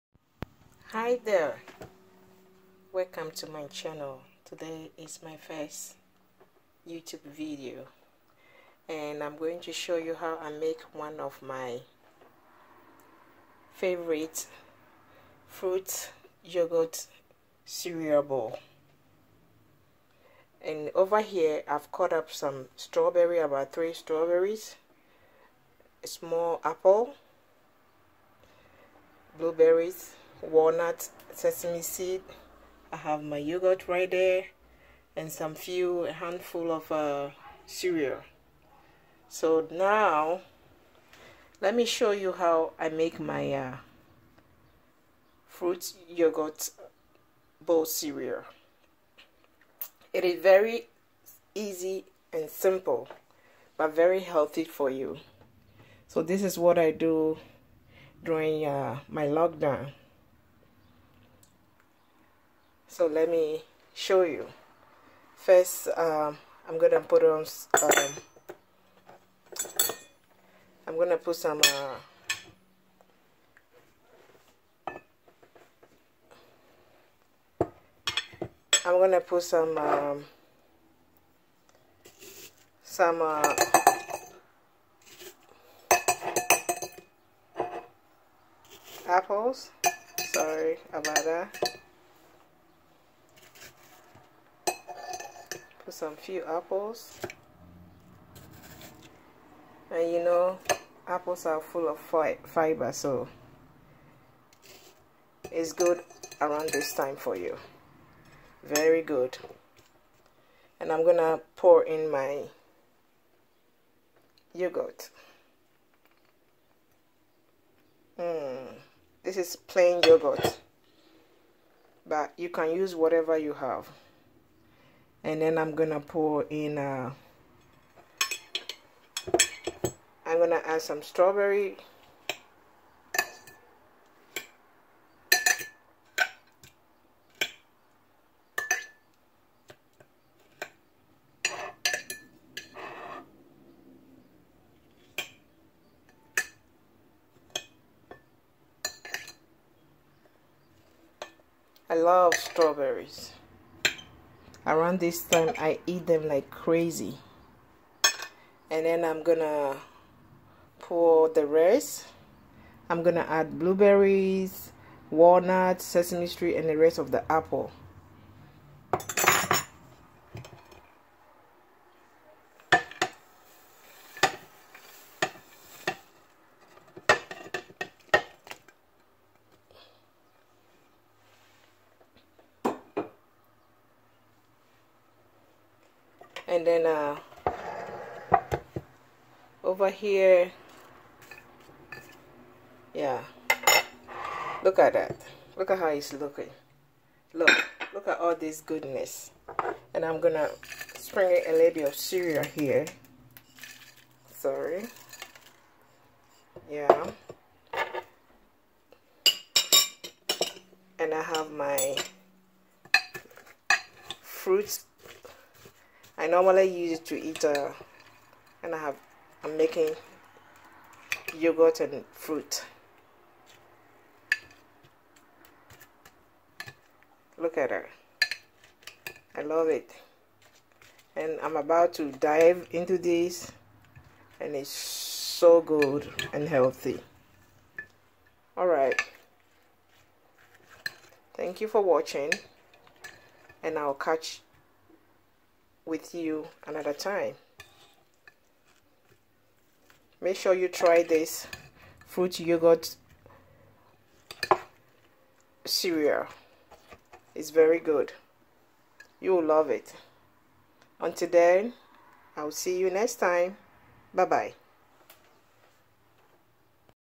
Healthy Fruits And Yogurt Cereal Bowl During Lockdown.
Today, I'm sharing with you one of my favorite Fruits,seeds,granola cereal and Yogurt bowlThis recipe is healthy, delicious,
and very simple  to follow.The dices apple,walnuts and sesame seeds give a nice
 crunch bite while the strawberry and the yogurt just melt in your mouth.